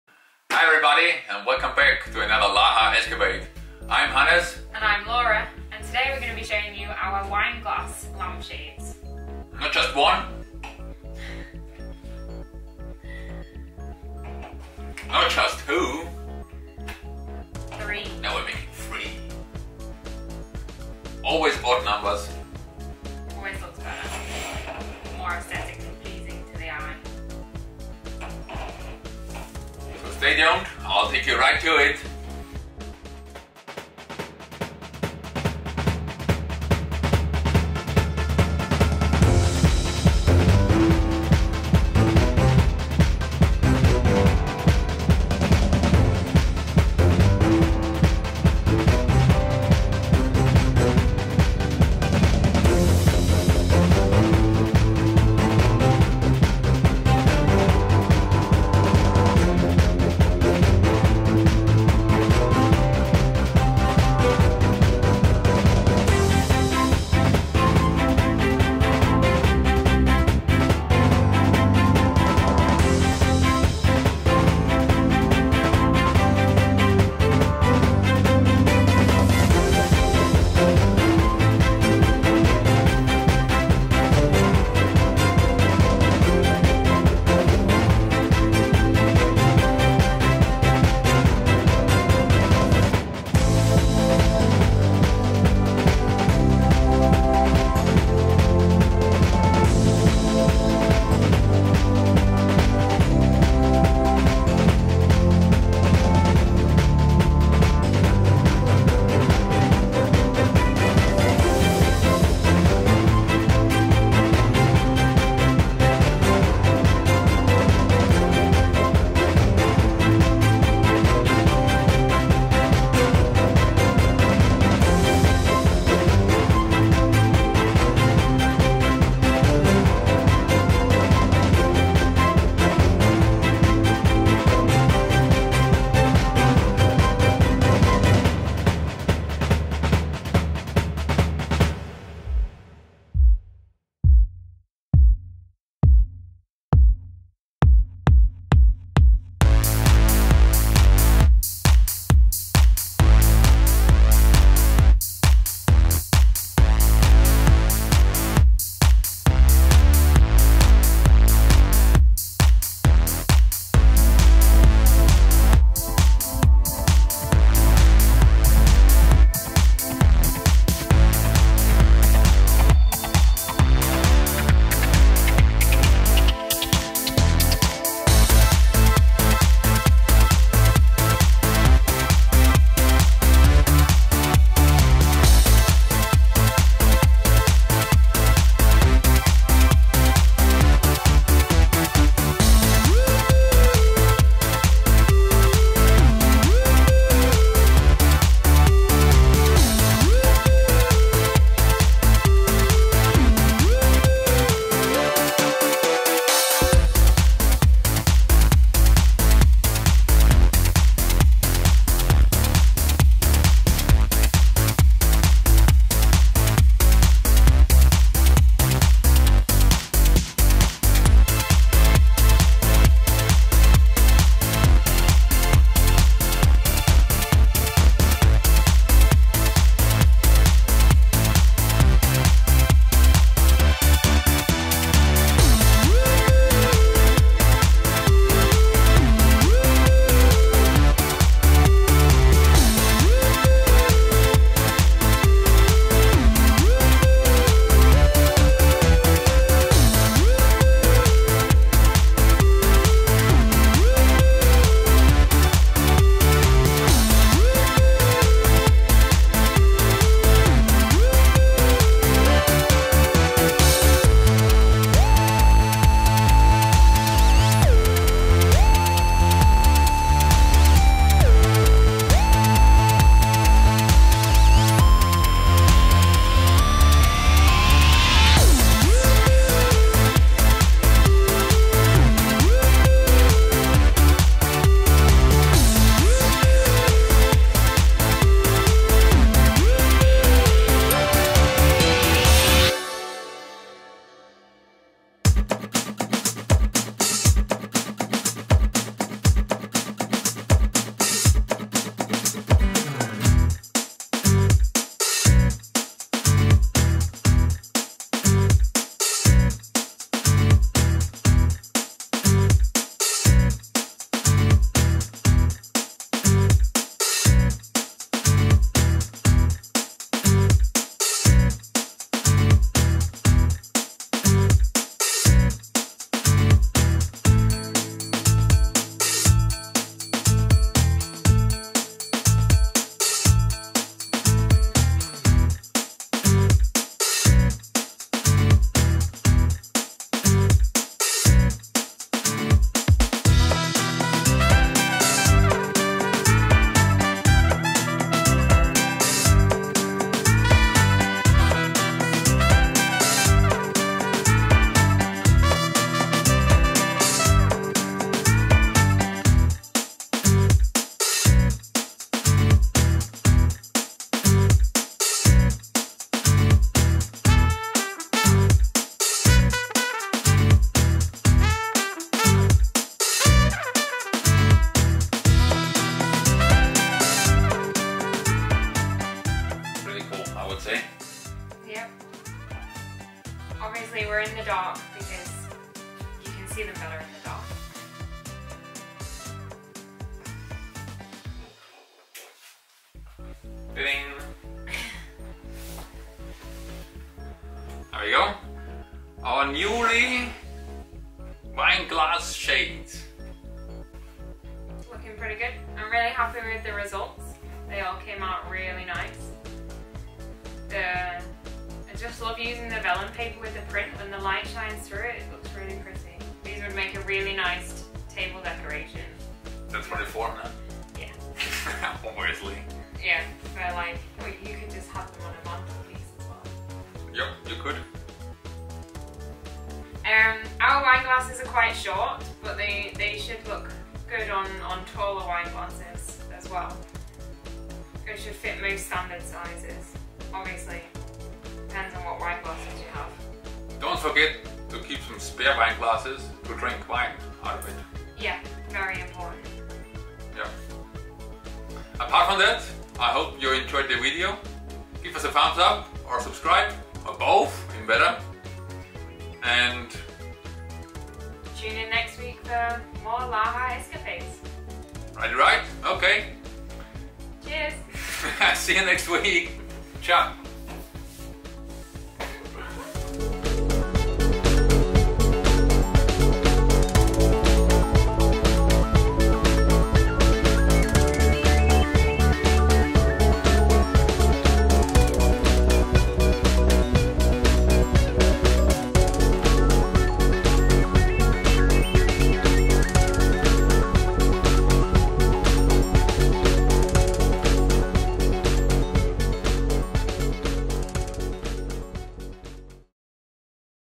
How to make wine glass shades with a LaHa wood print.
In this escapade we show how we make our wine glass shades with one of our prints. This print is from an ash tree which grow just behind our shed, unfortunately we had to cut it down due to a fungus disease.